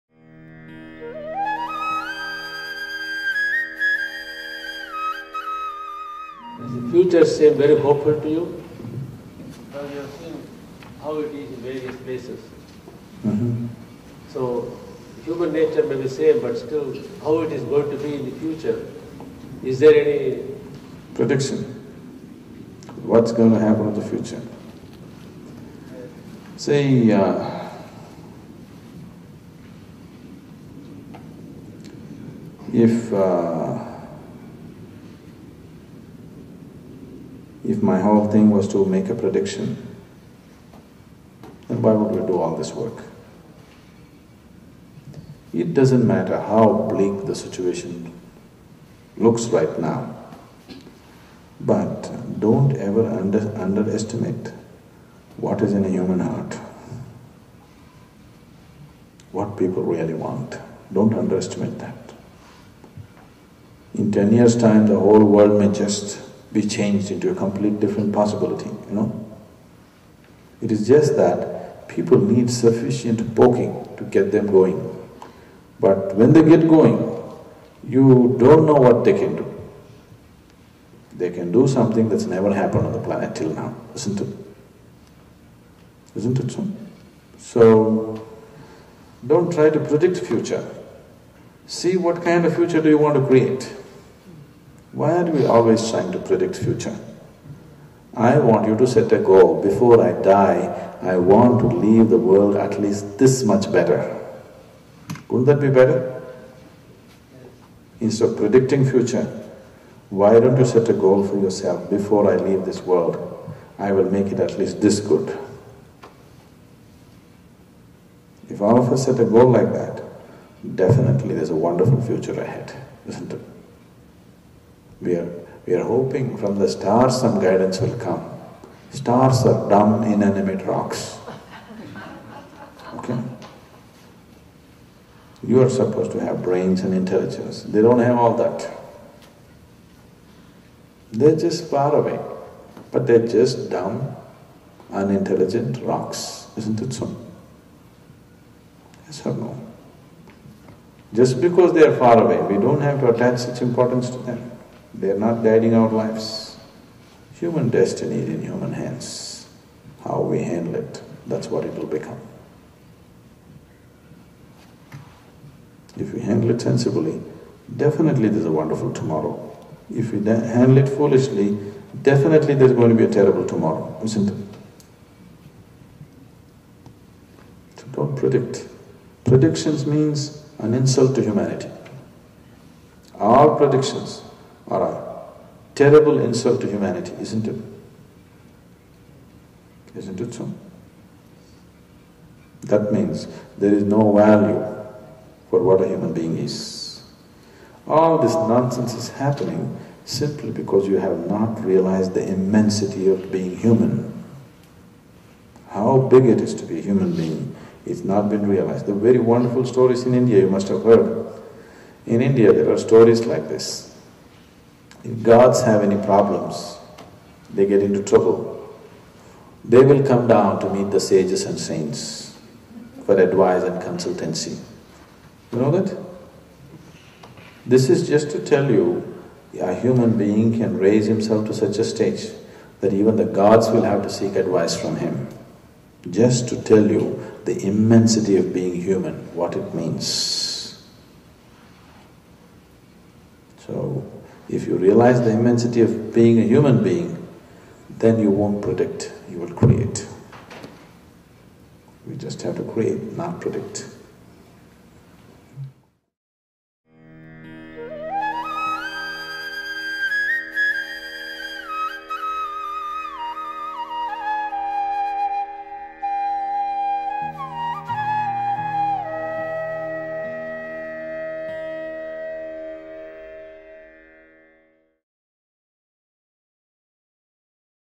What Will Happen in the Future? | Sadhguru
Responding to a question about the future of humanity, Sadhguru explains that we should never underestimate the human heart. In ten years' time, the whole world may just be changed into a completely different possibility. People are going by predictions simply because they have not realized the immensity of what it means to be human.

Sadhguru Talks @ IE Program